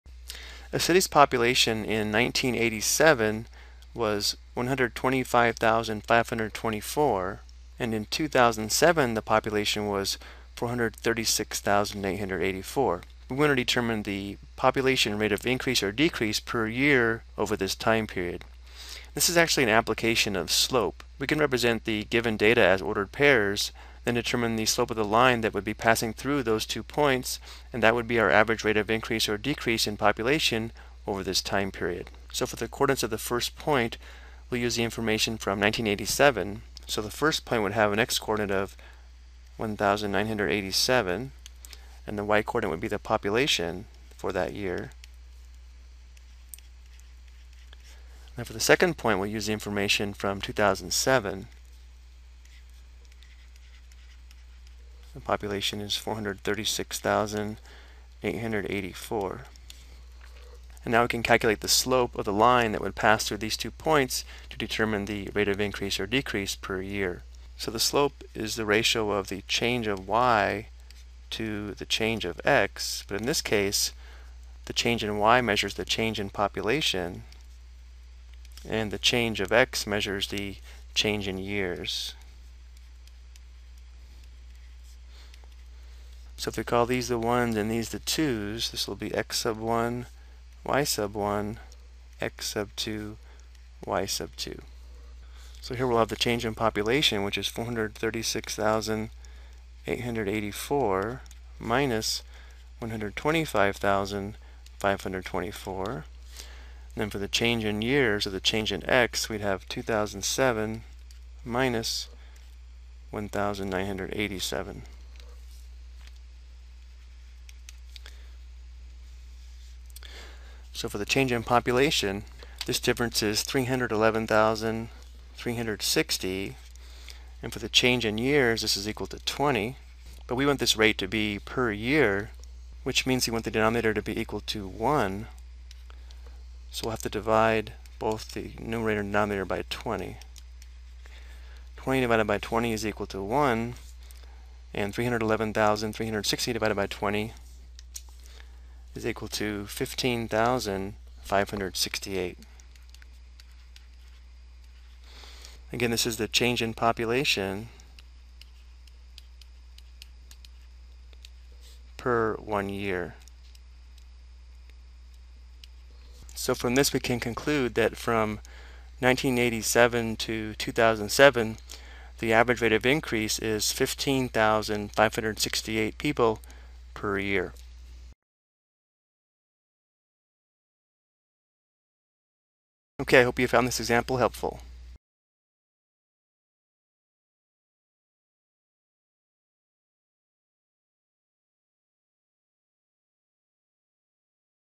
Ex: Slope Application Involving Population Growth
This video provides an real life application of the slope of a line to determine the average rate of population growth per year over a period of time.